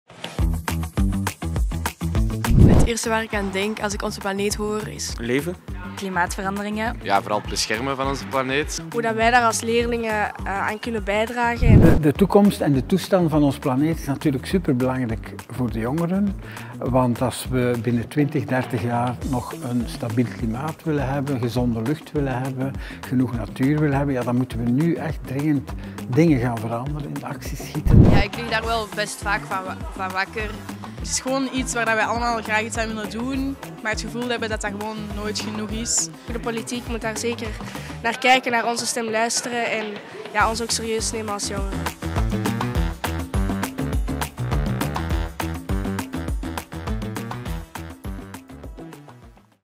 A1000 thema onze planeet
Studenten journalistiek van de AP Hogeschool kwamen langs bij de schoolworkshops van de A1000. Ze gingen samen met leerlingen en experten in gesprek over de verschillende thema's. Deze video gaat over het thema 'onze planeet'.